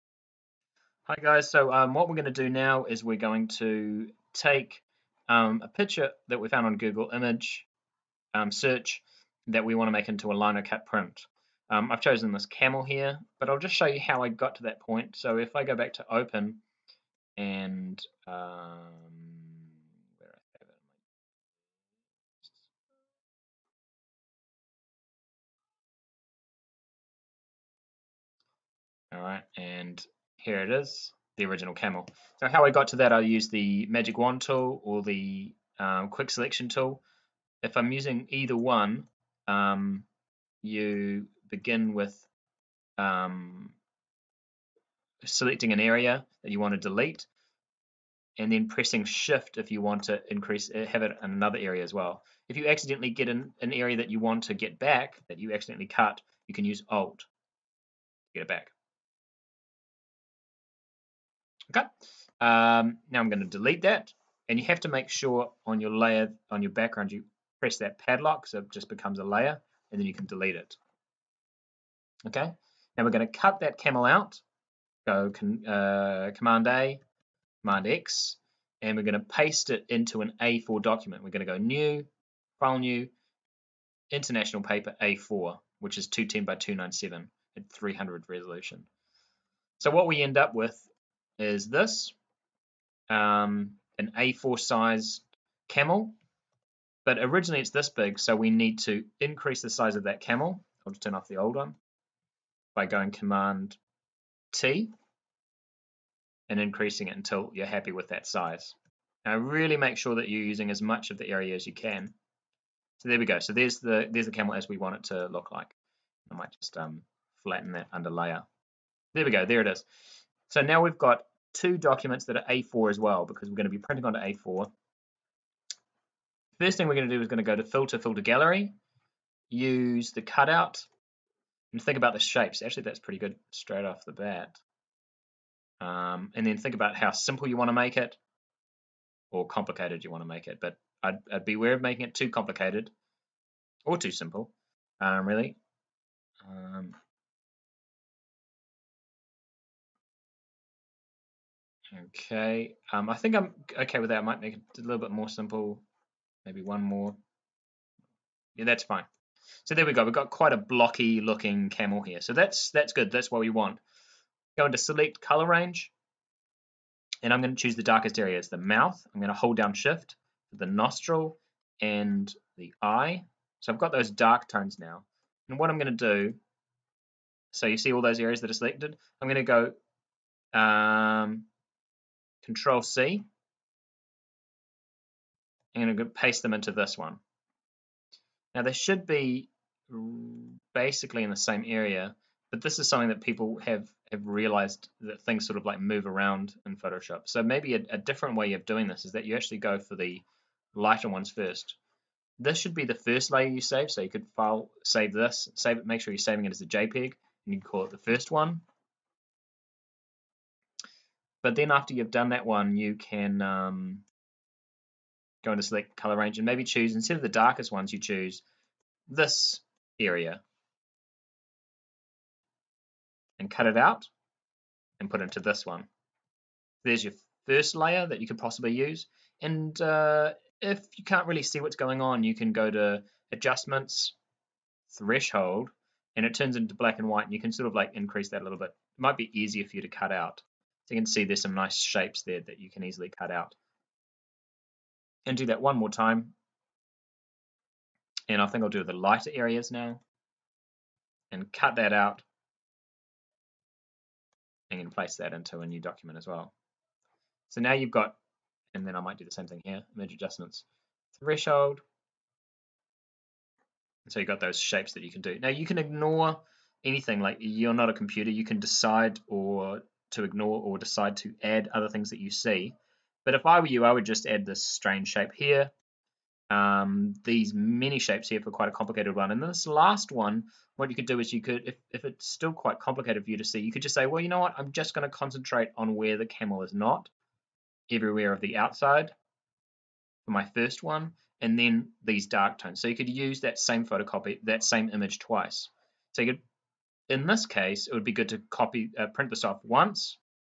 How to Separate layers in Photoshop for Linocut print
Year 10 linocut print instructions... using Google Image search, Adobe Photoshop.